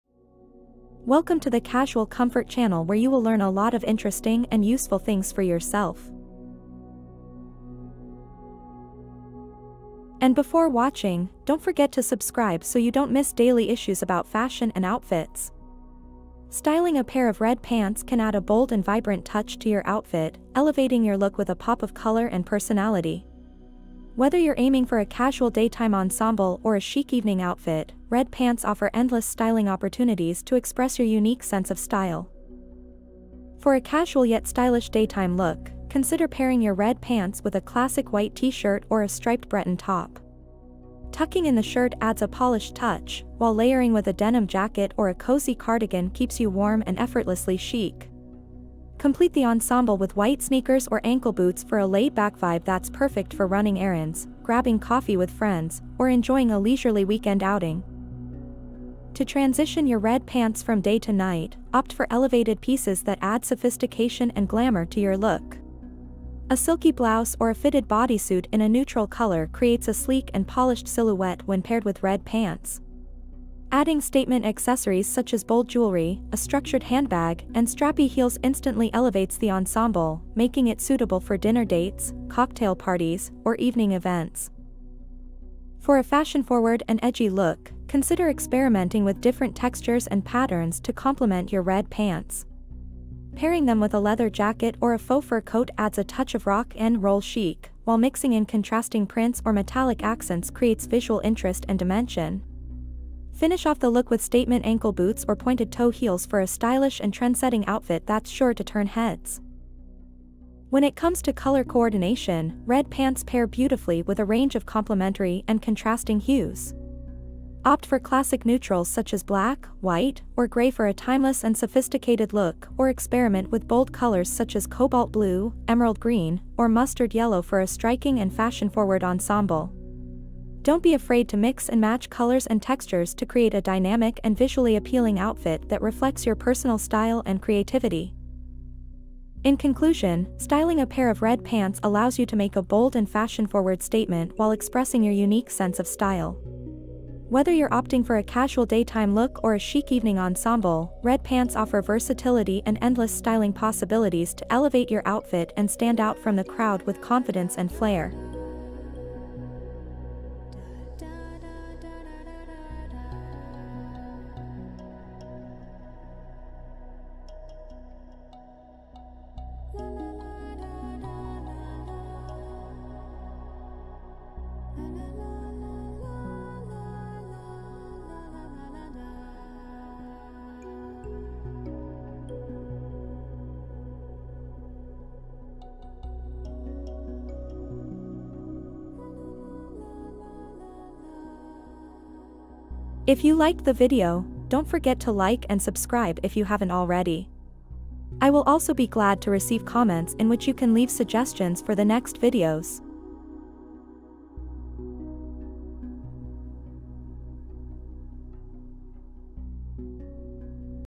Stylish Red Pants Outfit Ideas: Elevate Your Look with Bold Elegance
Explore the world of fashion with our curated collection of stylish red pants outfit ideas. Red pants are a versatile wardrobe staple that can instantly elevate any look with their bold and sophisticated appeal. In this video, we'll guide you through a variety of chic and trendy outfit combinations featuring red pants, perfect for any occasion. From casual outings to formal events, discover how to effortlessly incorporate this statement piece into your wardrobe. Whether you're aiming for a bold and daring ensemble or a more understated and elegant look, there's something for everyone in this fashion-forward showcase. Get inspired and unleash your inner fashionista with our stylish red pants outfit ideas!